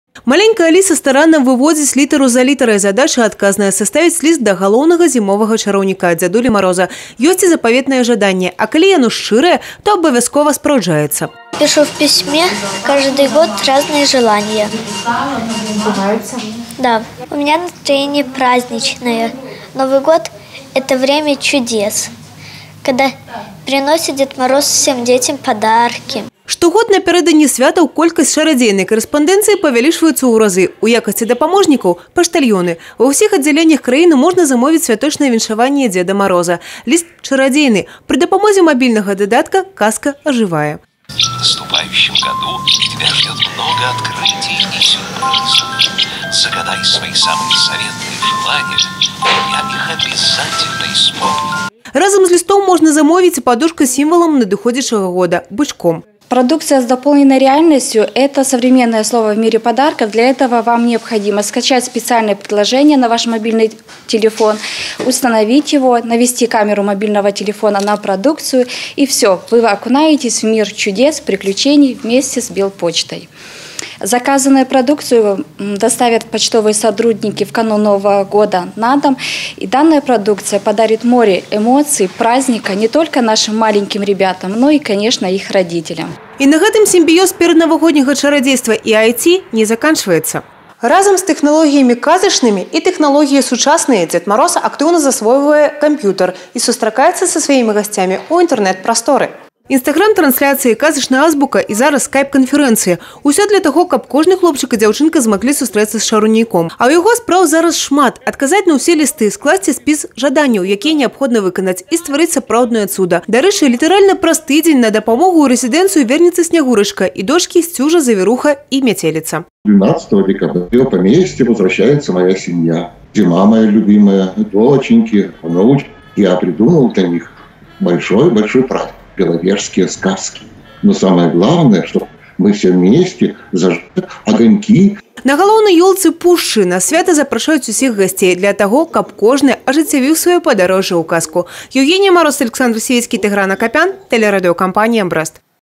З ВЕРАЙ У ЧАРАЎНІЦТВА (Відэа)
4-га снежня ў свеце адзначылі дзень заказаў падарункаў і напісання лістоў Дзеду Марозу. Напярэдадні Новага года пошта чараўніка працуе ў актыўным рэжыме. Колькасць карэспандэнцыі павялічваецца да 30 тысяч лістоў. А зараз пушчанскі чараўнік выходзіць і ў анлайн.